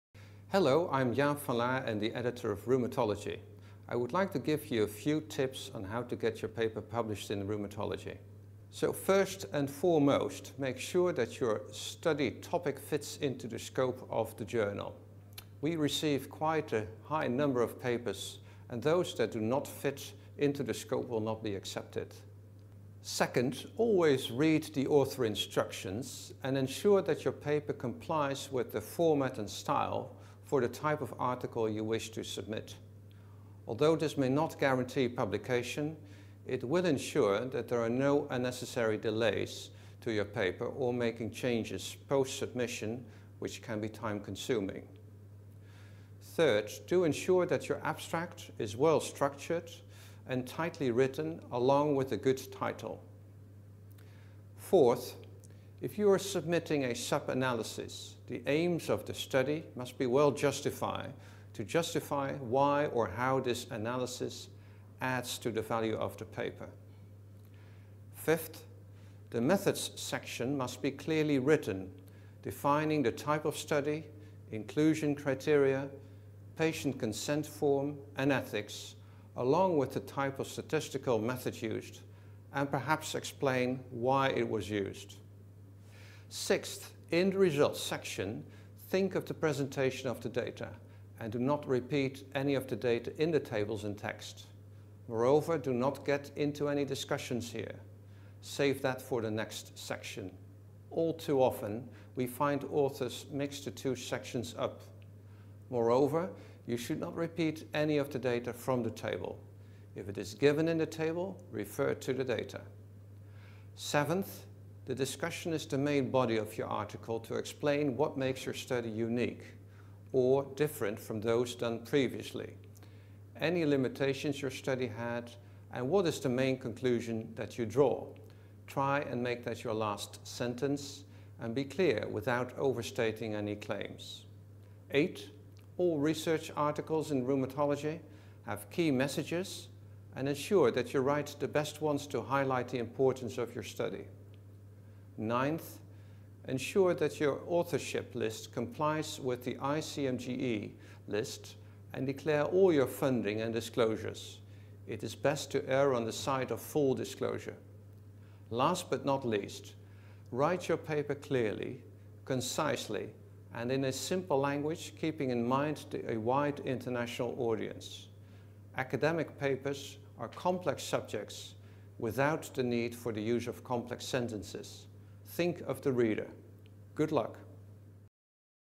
Top Tips on Getting Published in Rheumatology (Journal)
Rheumatology Editor Jaap van Laar gives his top ten tips for getting papers published in the journal, including what his editorial team are looking for in a manuscript.

Professor Jaap van Laar obtained his MD and PhD from the University of Leiden, The Netherlands. He has worked at Leiden University, The Netherlands; the National Institute of Arthritis and Musculoskeletal and Skin Diseases, USA and Newcastle University, UK. His research focus is on rheumatoid arthritis and systemic sclerosis, investigating novel treatment paradigms to improve patient outcomes.

Find out more about submitting your paper to Rheumatology by viewing the instructions for authors